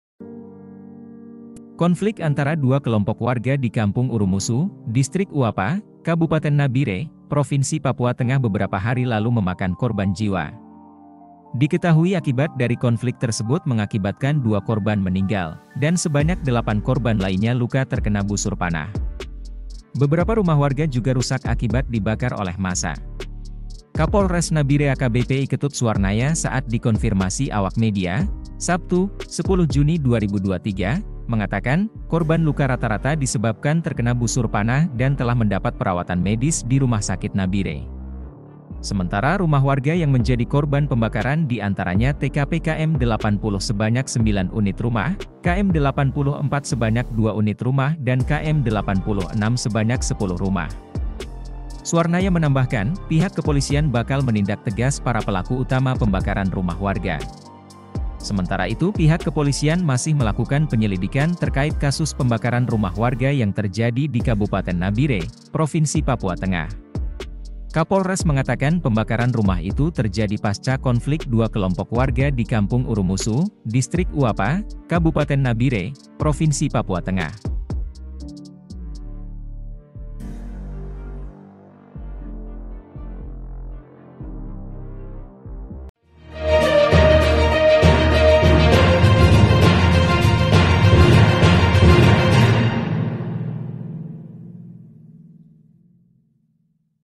Konflik Dua Kelompok Warga di Nabire Pecah akibatkan Dua Korban Meninggal
SELENGKAPNYA DI TAUTAN INI :
 Konflik antara dua kelompok warga di Kampung Urumusu, Distrik Uwapa, Kabupaten Nabire, Provinsi Papua Tengah beberapa hari lalu memakan korban jiwa.

Diketahui akibat dari konflik tersebut mengakibatkan dua korban meninggal,
dan Sebanyak 8 korban lainnya luka terkena busur panah.

Beberapa rumah warga juga rusak akibat dibakar oleh massa.

Kapolres Nabire AKBP I Ketut Suarnaya saat dikonfirmasi awak media, Sabtu (10/6/2023) mengatakan, korban luka rata-rata disebabkan terkena busur panah dan telah mendapat perawatan medis di Rumah Sakit Nabire.

*editor: jefri